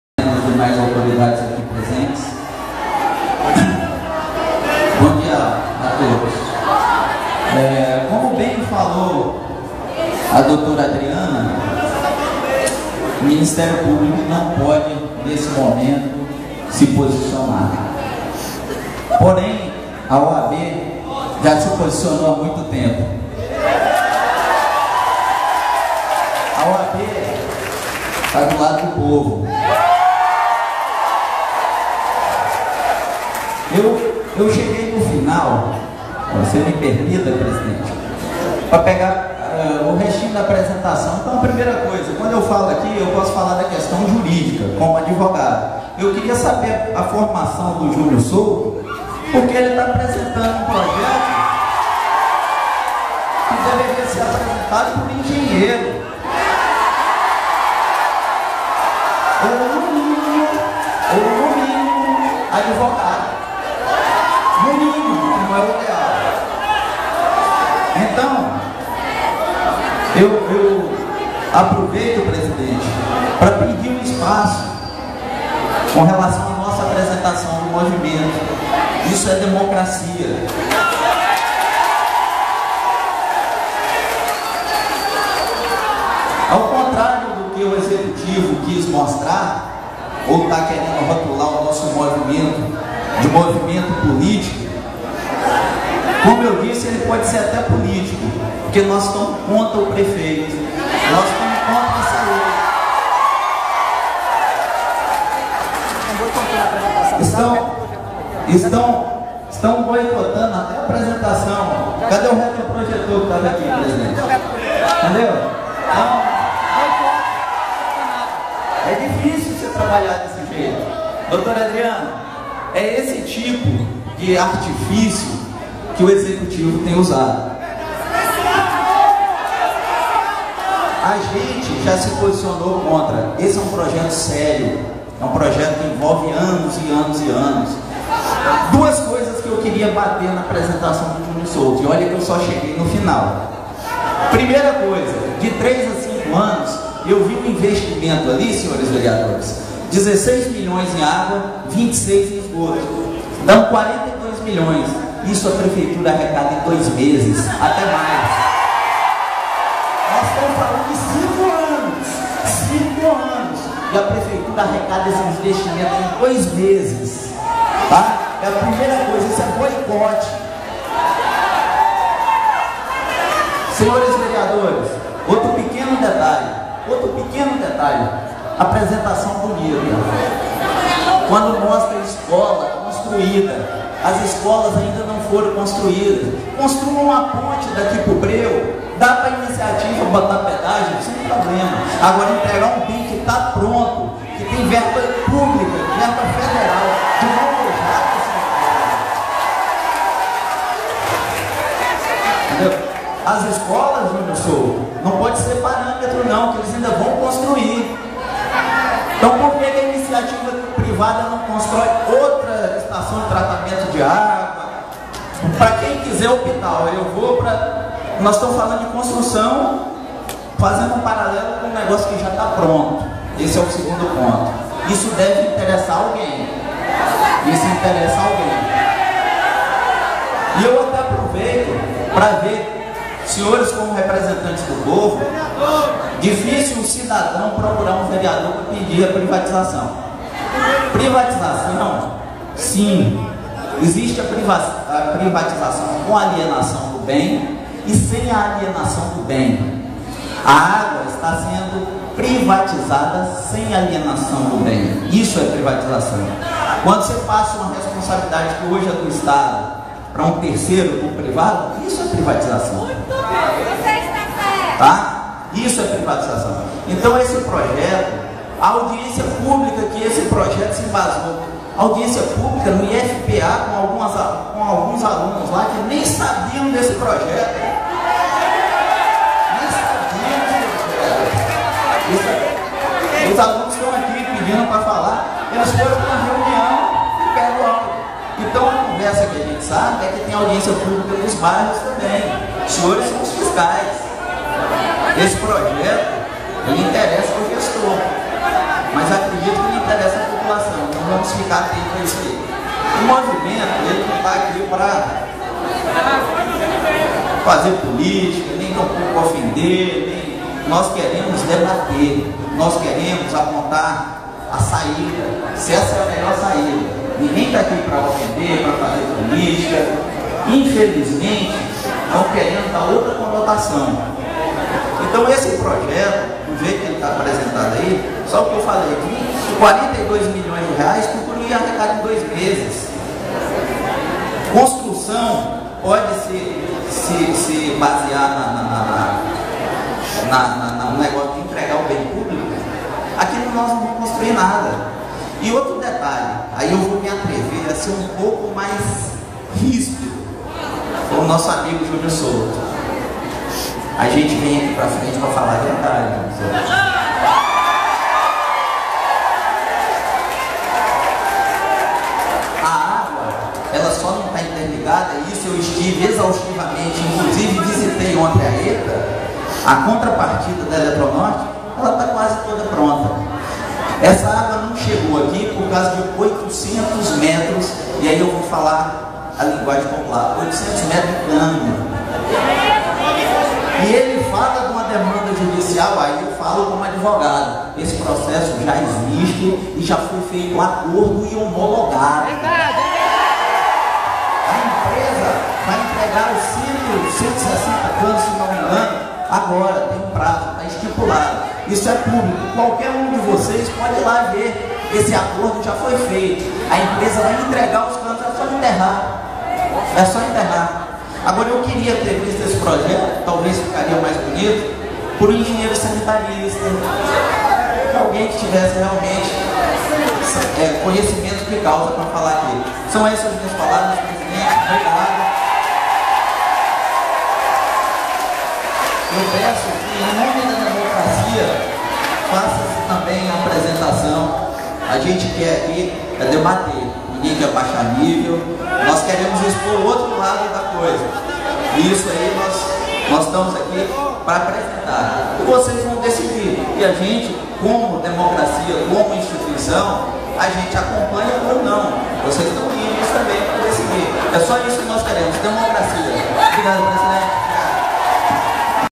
Presidente da OAB é contra a Privatização da Nossa Água
Durante a Audiência Pública o Presidente da OAB se manifesta contra a Privatização da Nossa Água e contra o entreguismo do prefeito e dos vereadores de Tucuruí